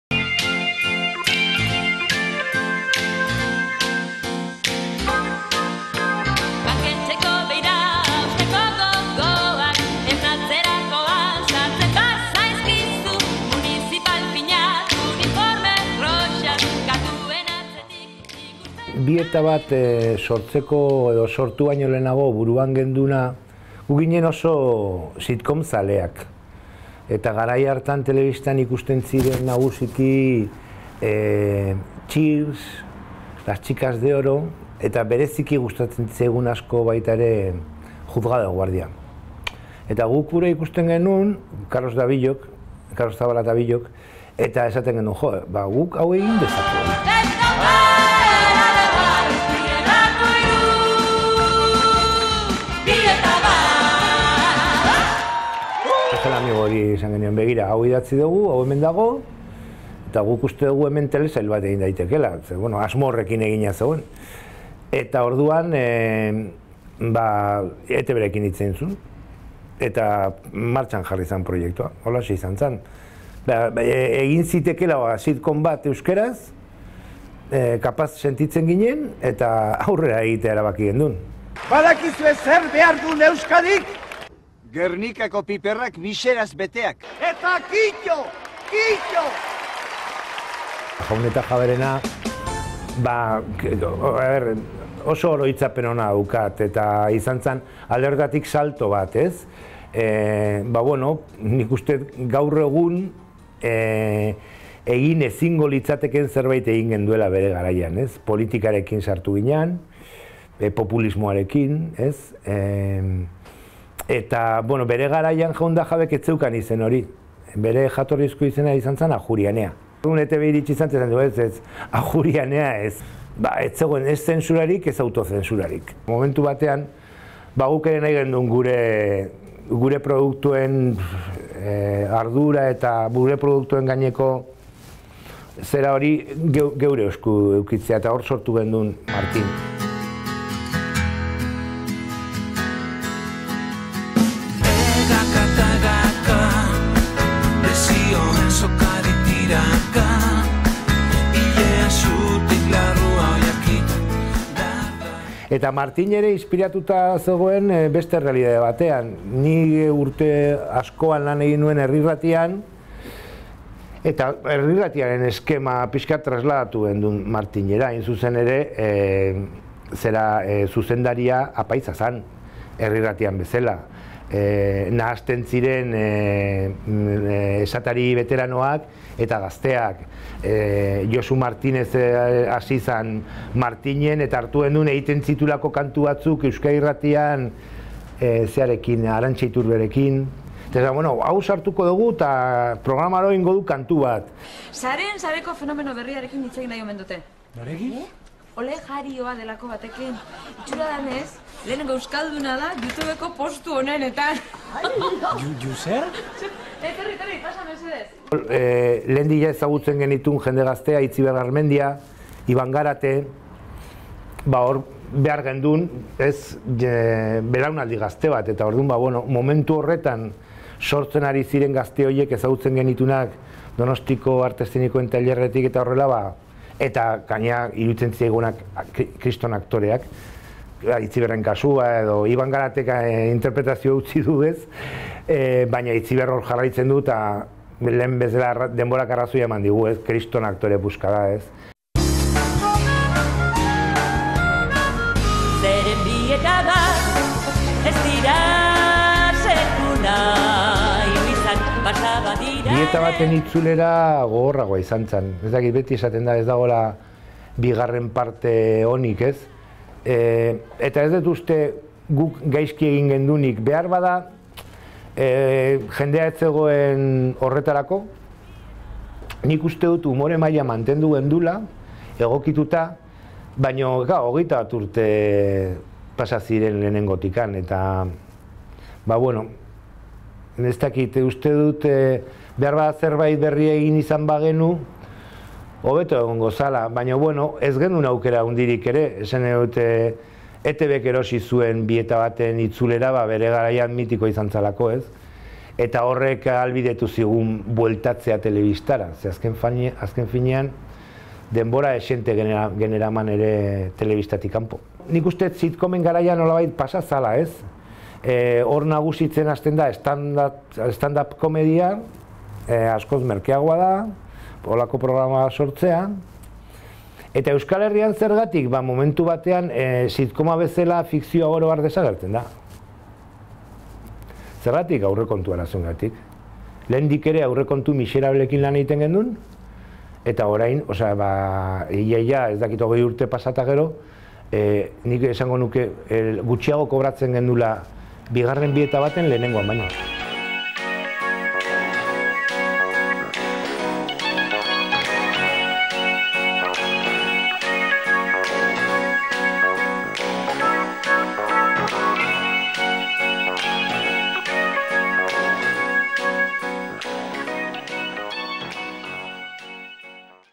Euskarazko telesailek 30 urte (II): Sitcomak
| Egoerazko komediak: 'Bi eta bat' eta beste hainbat. Eneko Olasagasti gidoilari eta zuzendariari elkarrizketa